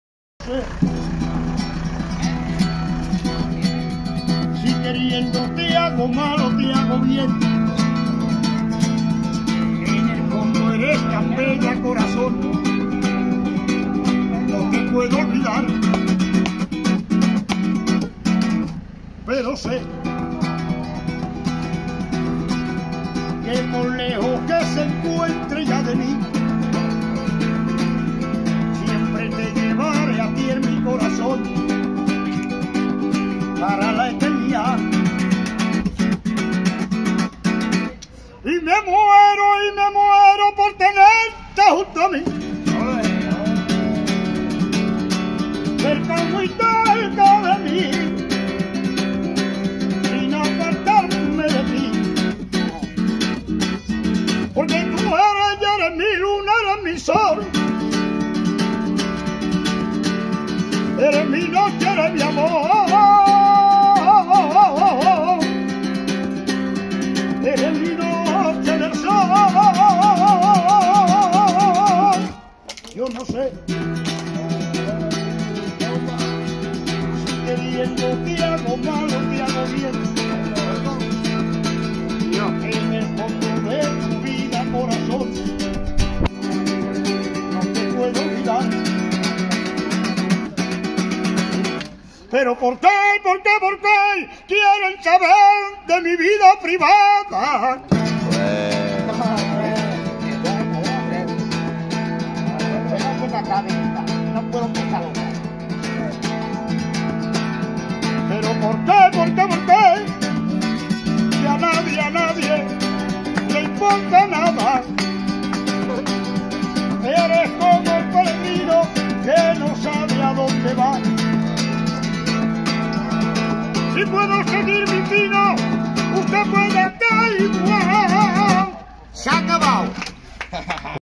Angel el Farina cantando en la palmilla... el mona
Gitano Angel... El farina malagueño cantando en la palmilla...